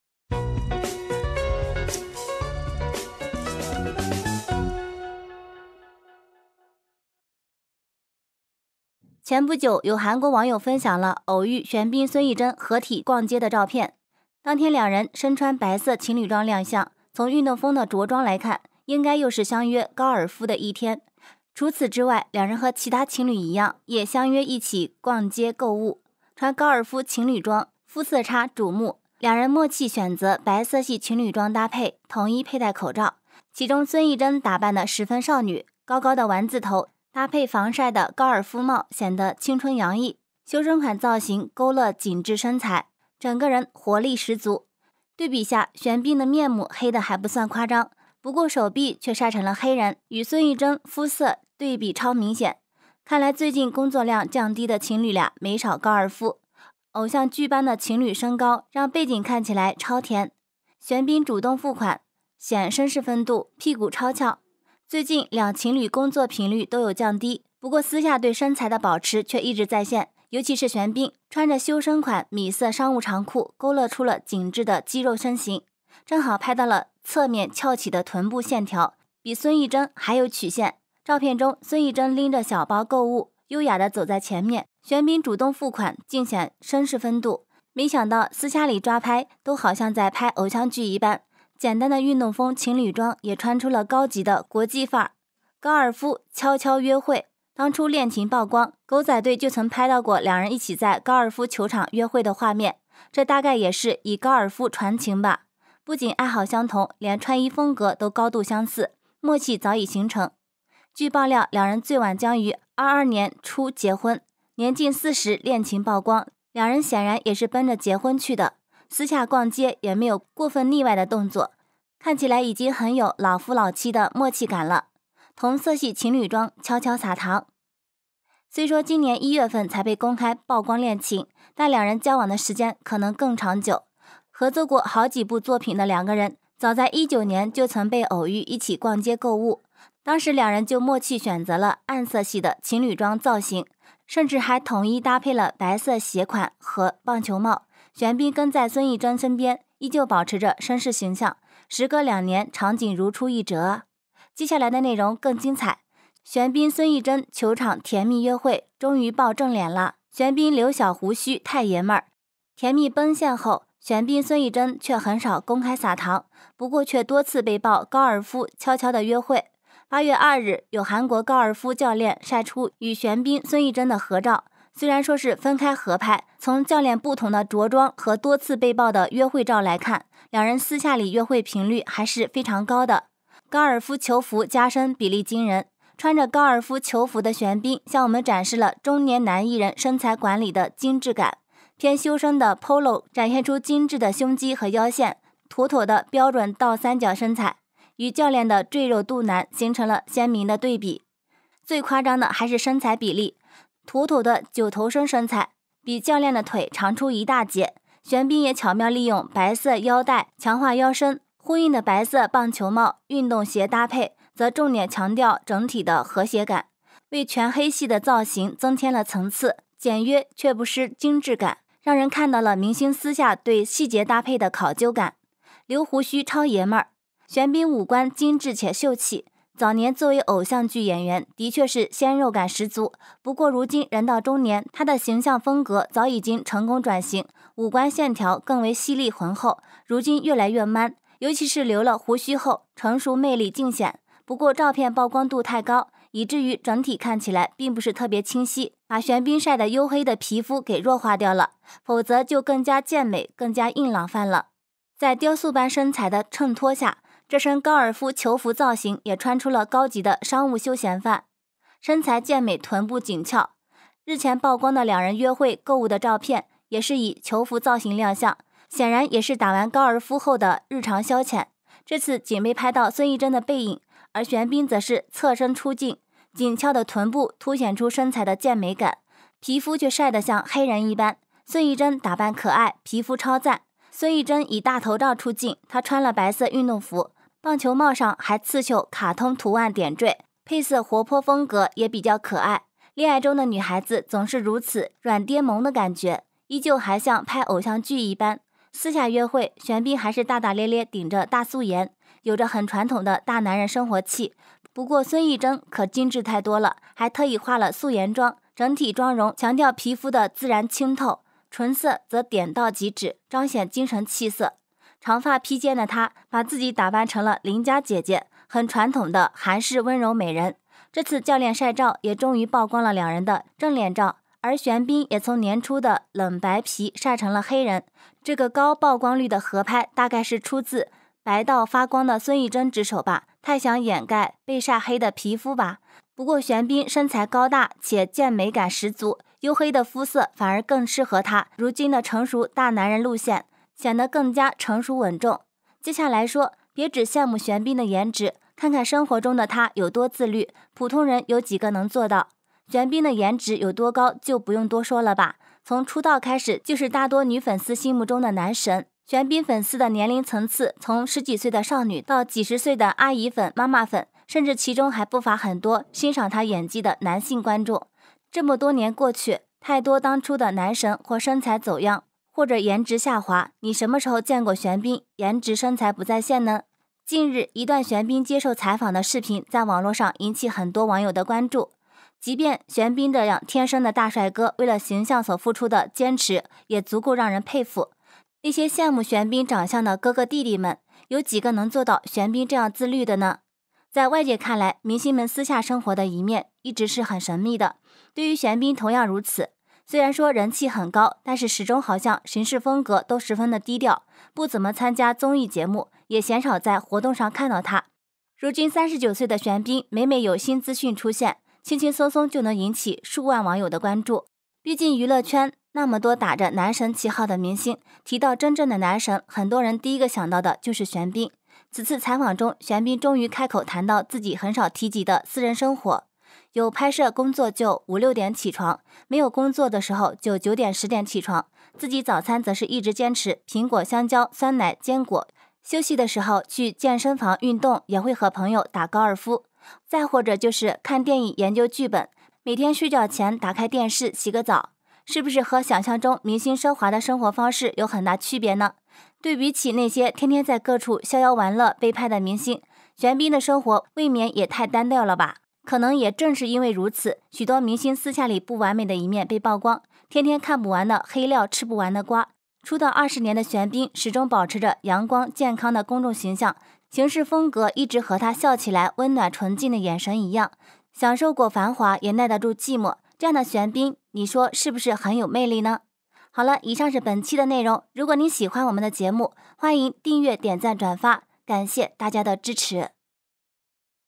玄彬孫藝珍甜蜜逛街，玄彬主動付款晒成「黑人」，屁股比孫藝珍還翹︱玄彬︱孫藝珍︱愛的迫降#華藝傳媒
玄彬孫藝珍甜蜜逛街，玄彬主動付款晒成「黑人」，屁股比孫藝珍還翹